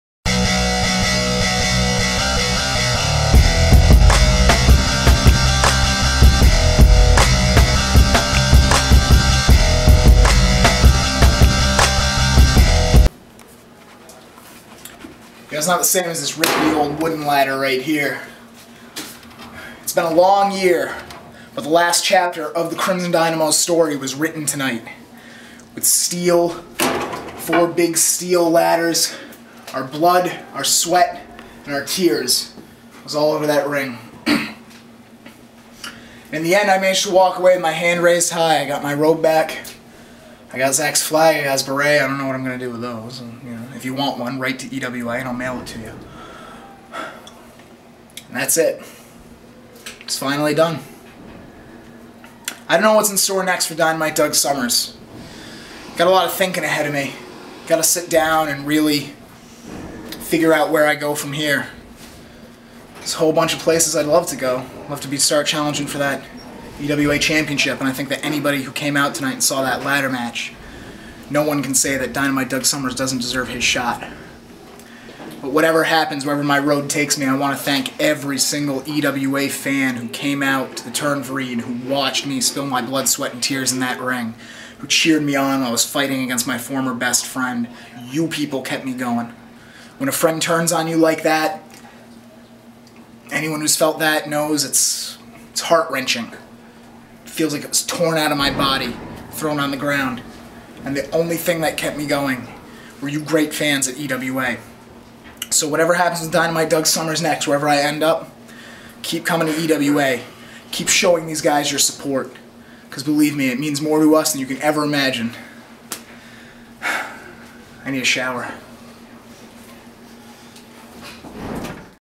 EWA "After The Match" 3-24-12: "Dynamite" Doug Summers
After months of battles between former best friends, Doug Summers finally got the better of Zachary Pierre Beaulieu. In an intense ladder match between former tag partners resulted in Summers winning and Beaulieu ending up in the stockade. Whats next for Summers?